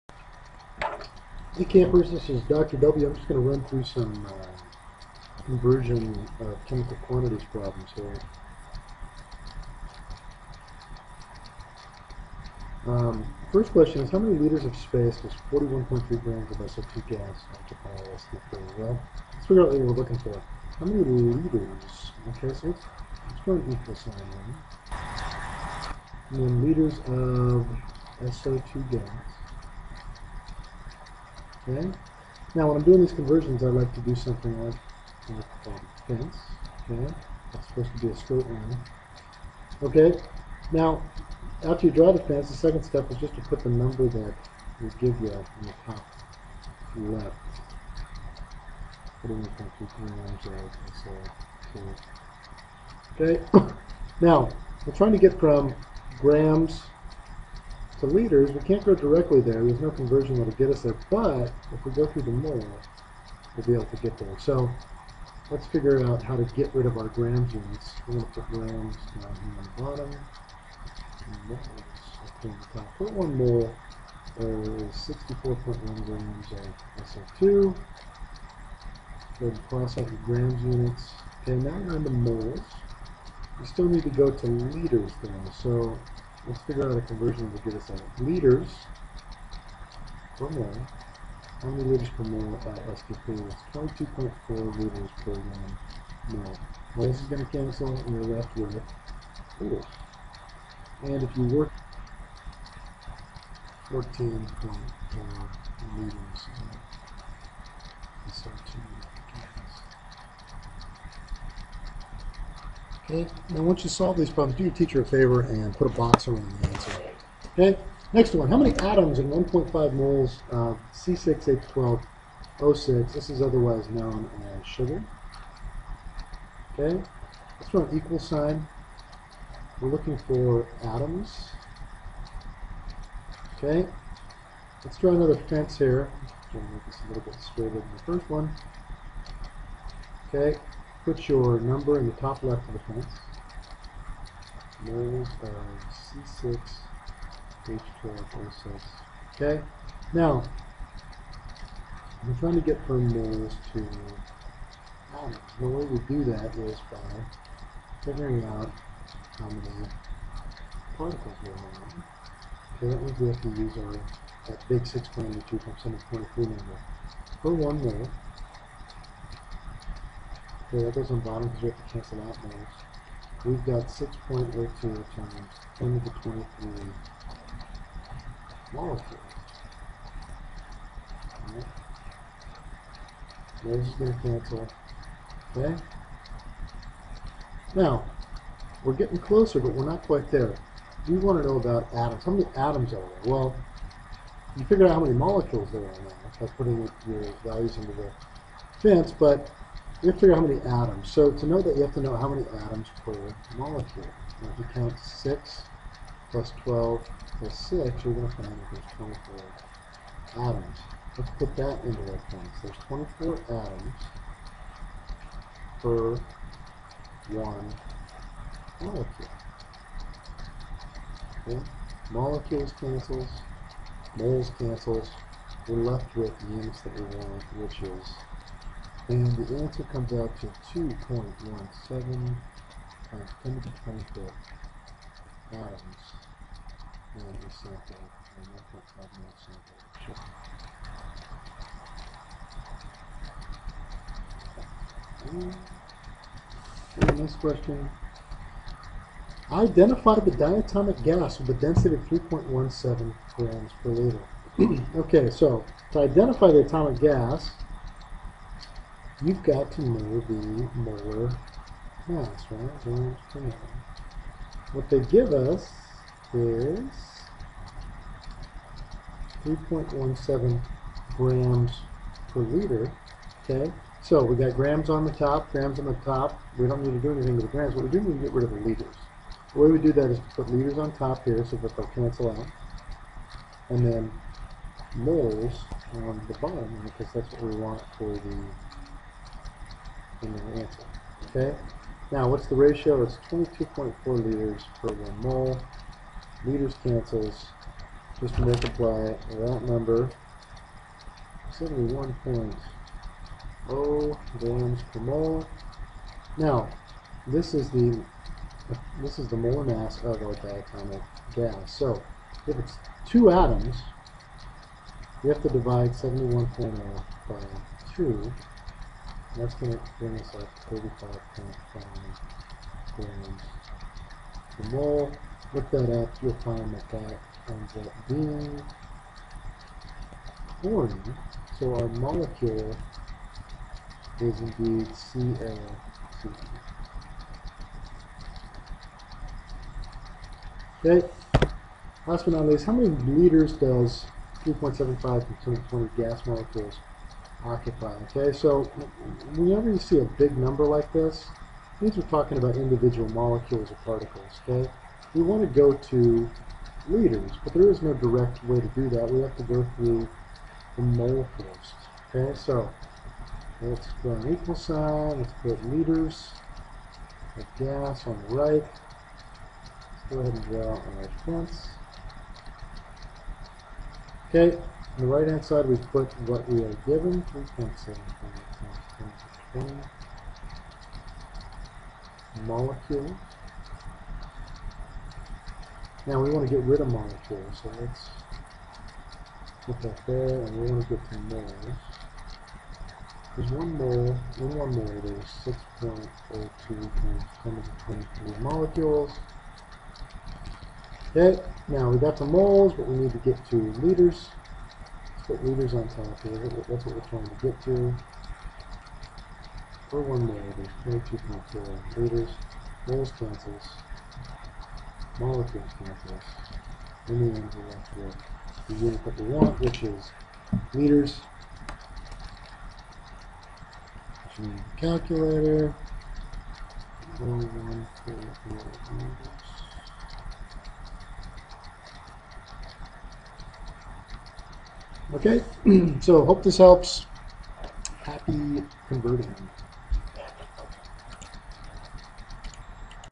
Chemical Unit Conversion Examples
Interested in science?

In this video...Dr. W works some examples of converting from grams to liters of gas, moles to atoms, molecules to liters and how to identify a gas from its density.  He utilizes the fence method (dimensional analysis) to solve the problems.